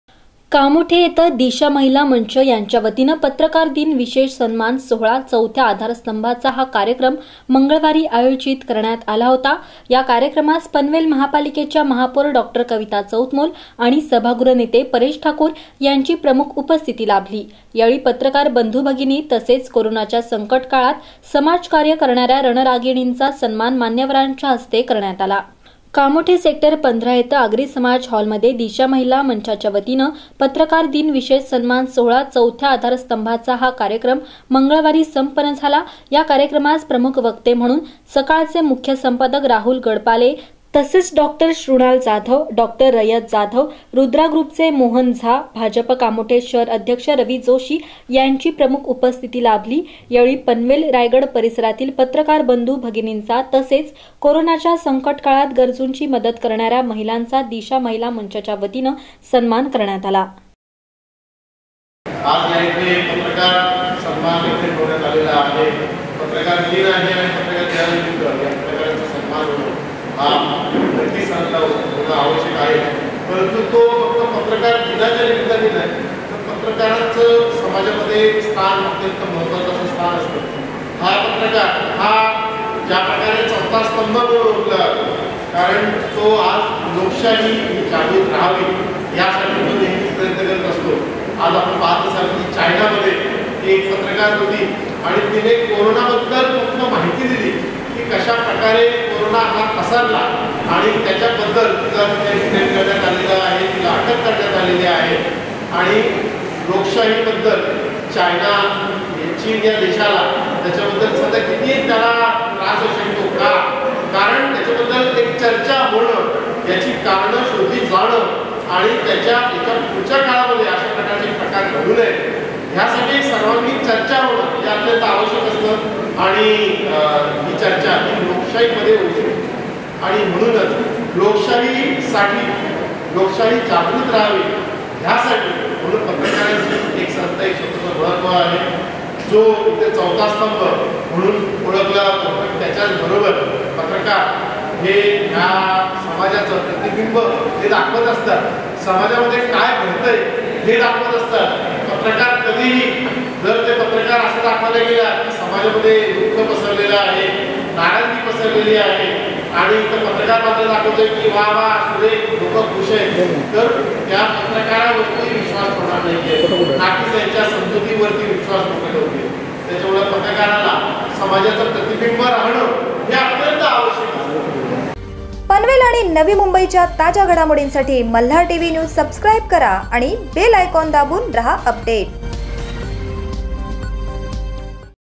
दिशा महिला मंच यांच्या वतीने पत्रकार दिन "सन्मान सोहळा - चौथ्या आधारस्तंभाचा" कार्यक्रमाचे आयोजन
दिशा महिला मंचच्यावतिने पत्रकार दिनाचे औचित्य साधून पनवेल महानगरपालिकेच्या महापौर कविता चौतमल, सभागृह नेते परेश ठाकूर यांच्या प्रमुख उपस्थितीत पनवेलमधील पत्रकारांचा पुरस्कार देवून गौरव करण्यात आला. यावेळी सकाळ वृत्तपत्राचे मुख्य संपादक राहुल गडपाले, डॉ. श्रूणाल जाधव, डॉ. रजत जाधव, मोहन झा आदींच्या उपस्थितीत हा सोहळा पार पडला.

दिशा महिला मंच म्हणजे ' ती ' च्या अस्तित्वाचं हक्काचं व्यासपीठ हे स्लोगन घेवून पनवेलमधील कामोठे परिसरात कार्यरत आहे. गेली ४ वर्षे पत्रकारिता क्षेत्रात कार्य करणाऱ्या पत्रकारांचा सन्मान या संस्थेमार्फत करण्यात येत असतो. मात्र गत वर्षी कोरोनाच्या महामारीत पत्रकार मात्र आपले कर्तव्य बजावत होते. त्यामुळे यावर्षीच्या या पुरस्काराला महत्त्व प्राप्त झाले होते. यावेळी विजय कडू, सानीप कालोते, विक्रम बाबर, हर्षल भदाणे पाटील, प्रवीण मोहोकर, तृप्ती पालकर, अरुणकुमार मेहेत्रे आदी पत्रकारांचा गौरव करण्यात आला.

यावेळी उपस्थितांना मार्गदर्शन करताना सभागृह नेते परेश ठाकूर यांनी बोलताना सांगितले की, मराठी भाषा शुद्धी आणि मराठी भाषेचा प्रसार जर कोण करीत असेल तर ते पत्रकार आहेत. ज्या पद्धतीने विनायक दामोदर सावरकर यांना २० वर्षांसाठी काळया पाण्याची शिक्षा झाली तरीही ते डगमगले नाही तर त्यांनी मराठी भाषा शुद्धीसाठी आपले जीवन खर्ची घातले. आणि आज त्यांचे काम पत्रकार करीत आहेत. त्यामुळे लोकशाही टीकविण्यासाठी ज्या पद्धतीने सावरकरांनी कार्य केले आहे. त्यापद्धातीने पत्रकारांचे सुरू असलेले काम नक्कीच भूषणावह आहे.